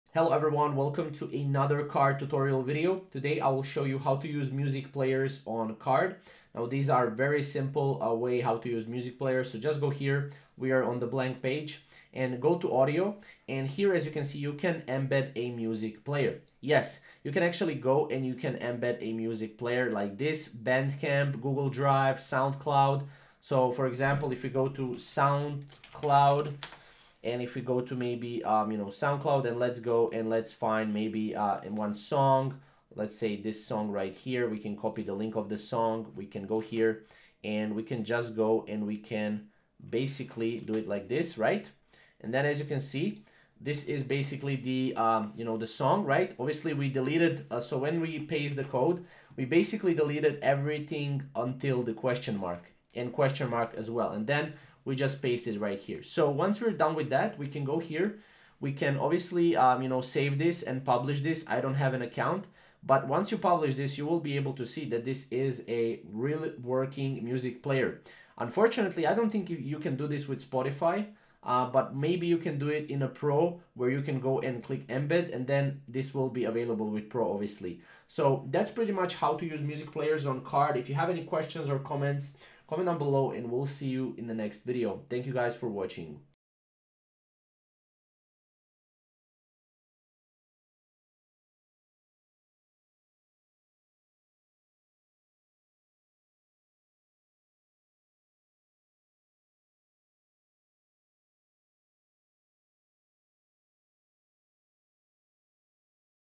How to Use Music Players on Carrd (2024)
In this video, I will show you how to use music players on carrd